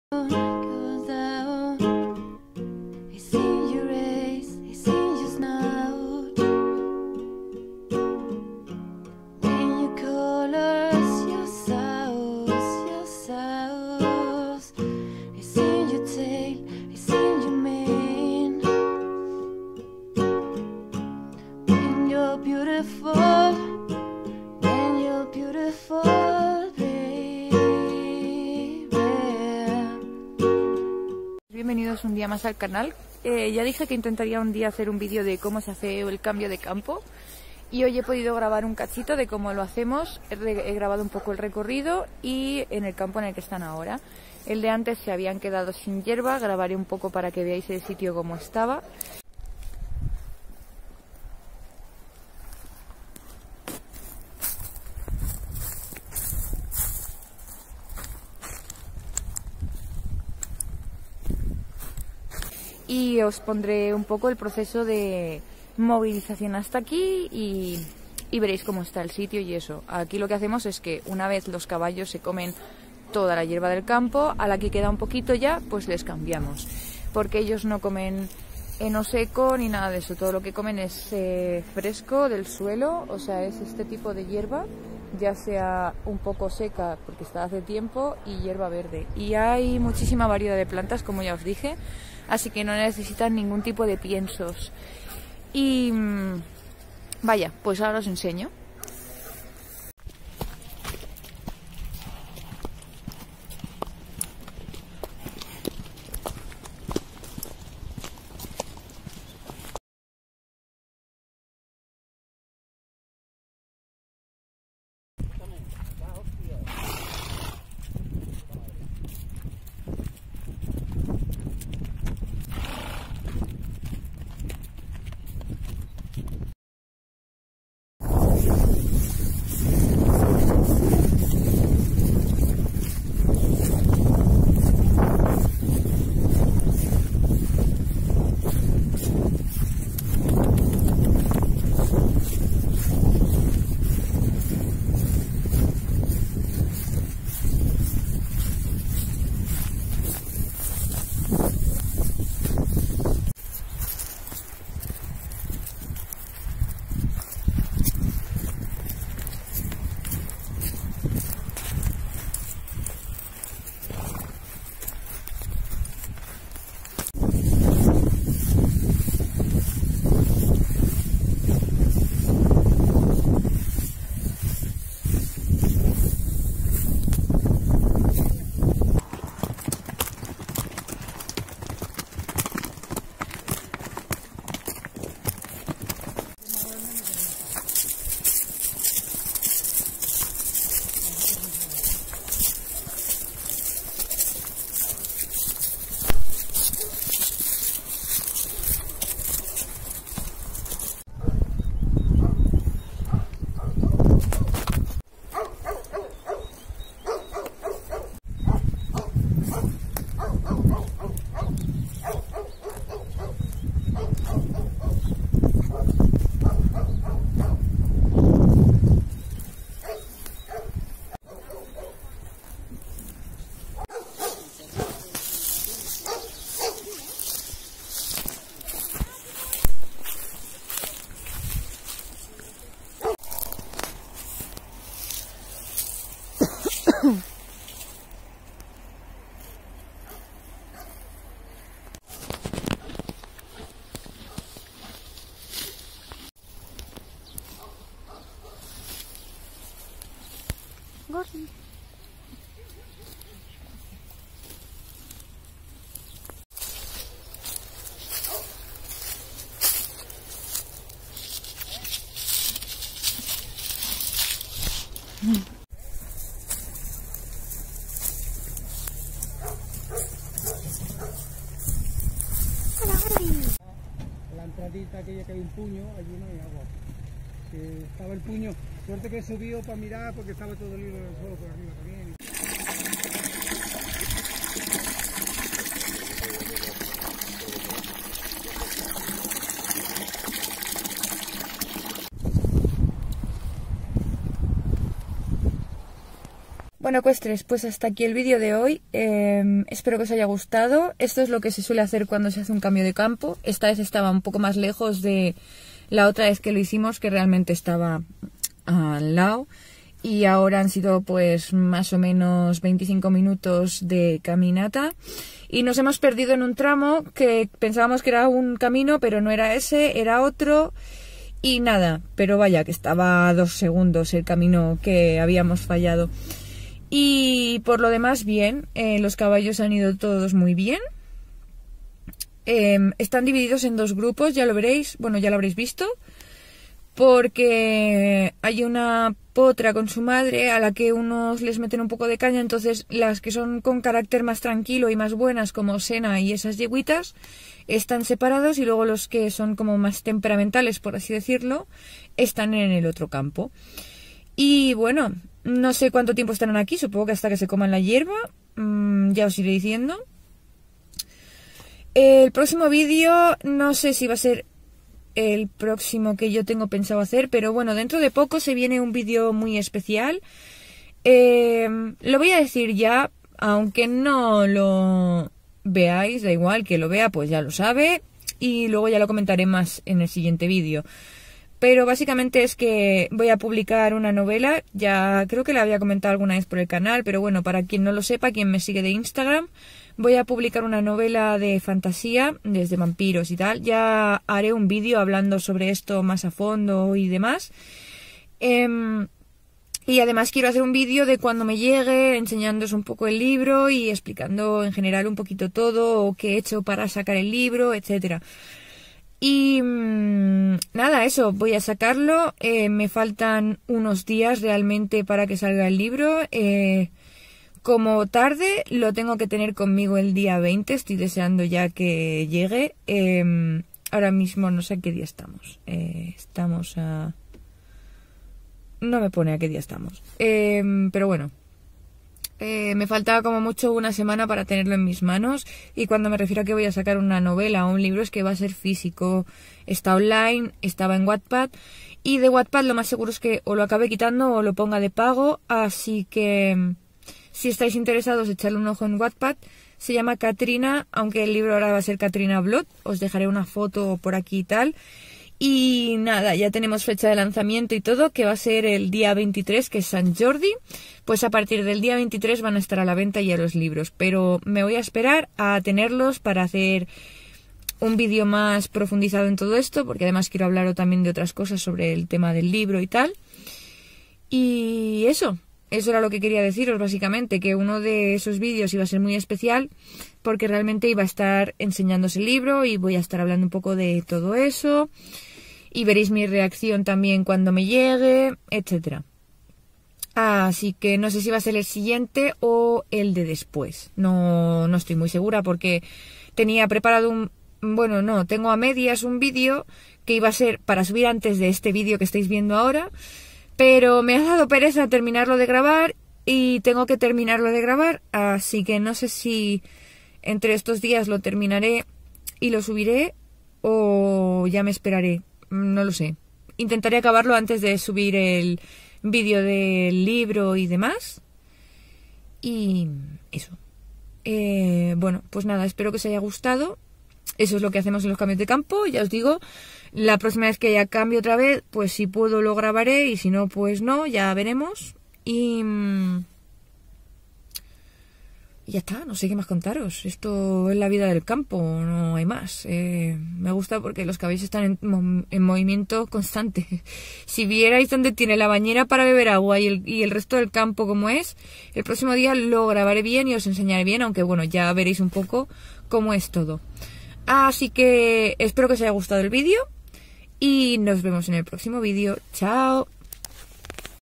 Cambiamos los Caballos de Campo 🐎 Noticias importantes!! horseblogg 5
Hola ecuestres! aquí os comparto un vídeo de cómo hacemos cambios de campo a los caballos 🐎❤️ y os cuento cositas importantes 😁
para más cositas y día a día podéis seguirnos en Instagram: